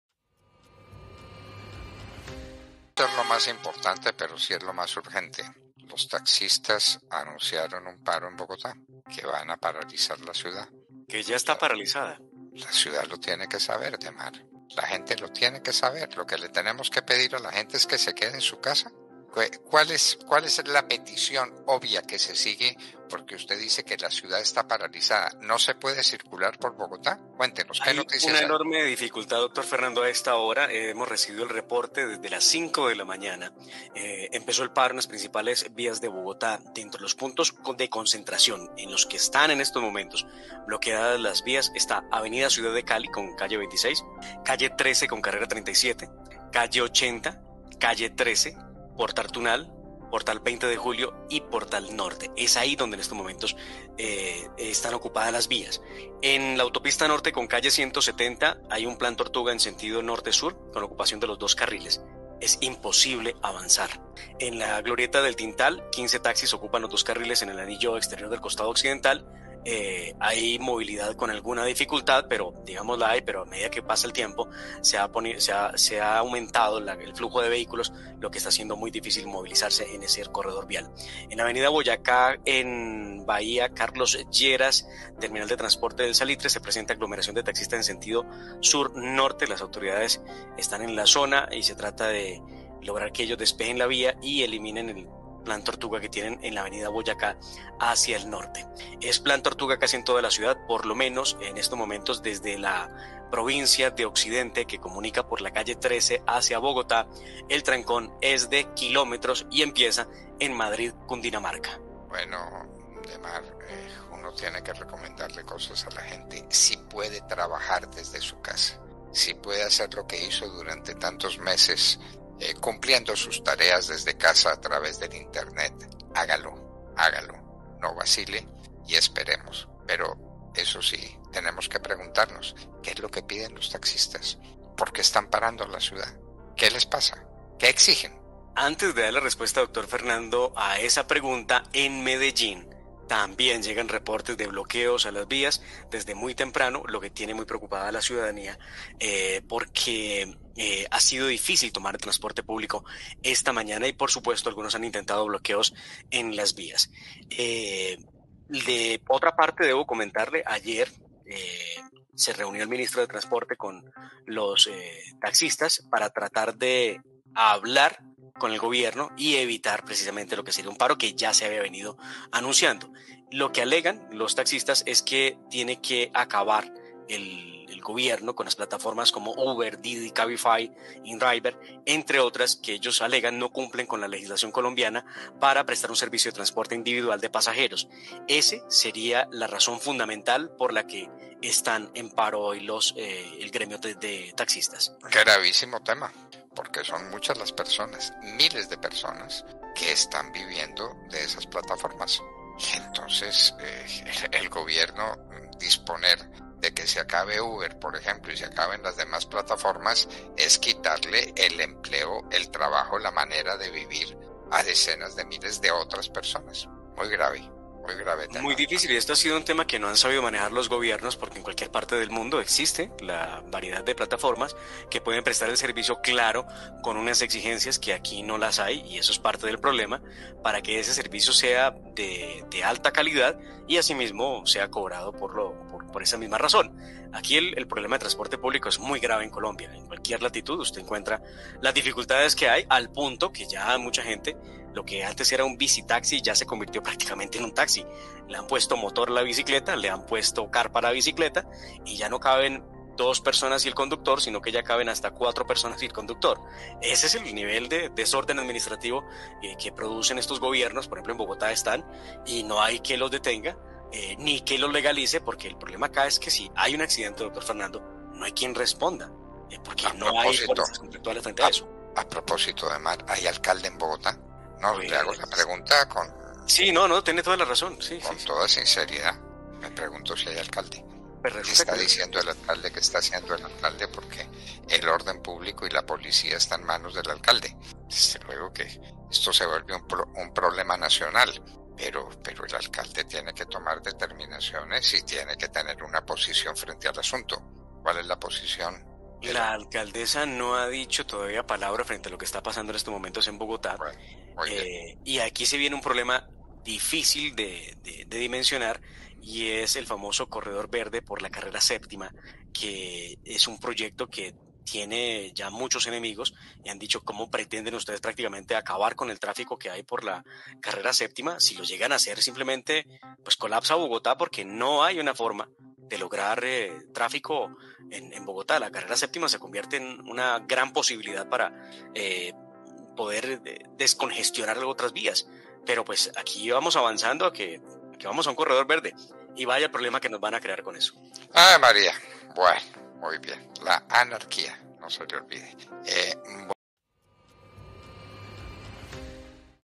NO HAY ALCALDES!!! 🎙️ CIUDADES DE COLOMBIA BLOQUEADAS POR TAXISTAS
Líderes taxistas se retiran de mesa de diálogo y reiteran el paro nacional del miércoles.